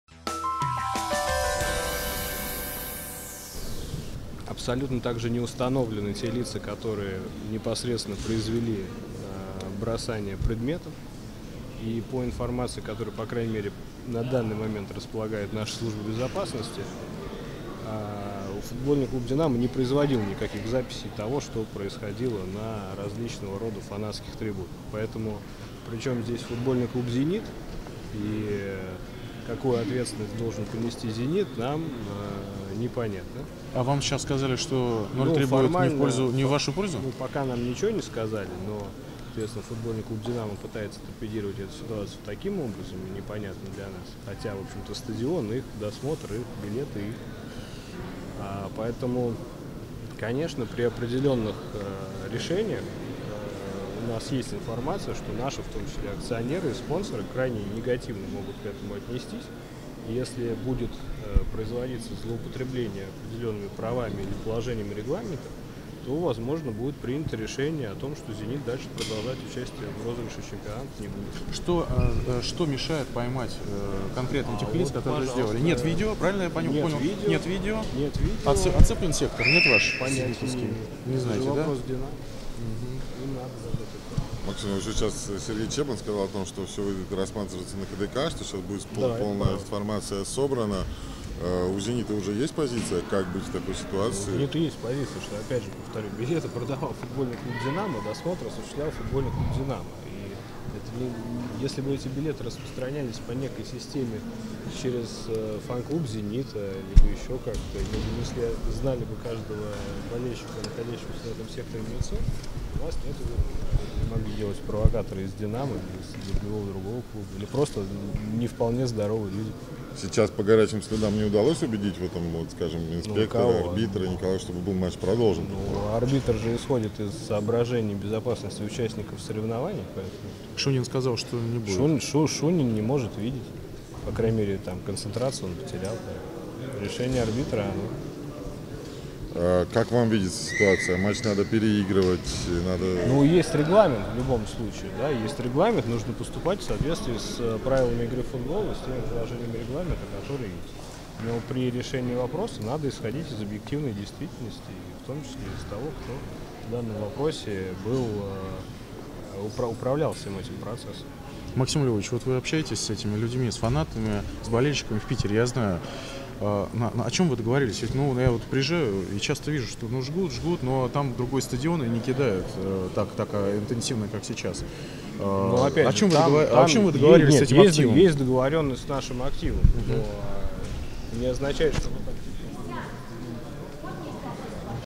Митрофанов: "Зенит" может сняться с чемпионата
Генеральный директор ФК "Зенит" Максим Митрофанов прокомментировал ситуацию с поведением болельщиков на стадионе в Химках.